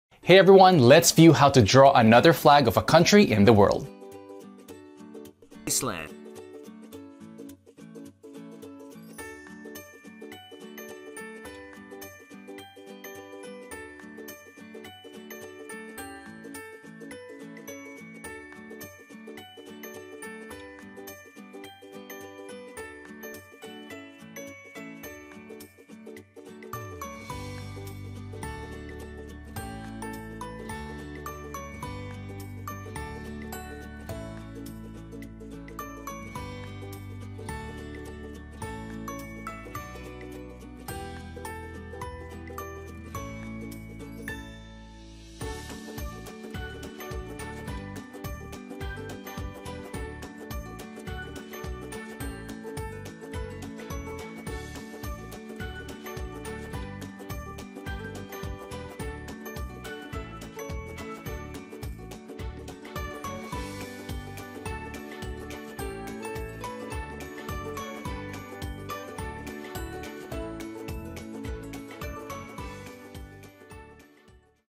Flag of Iceland - 🌍 How to Draw the Icelandic Flag - SVG Vector
- Flag of Iceland: Check out how to draw the flag of Iceland. Let’s discover how to draw the national flag of Iceland in a few minutes. Discover the national flags of all the countries in the world.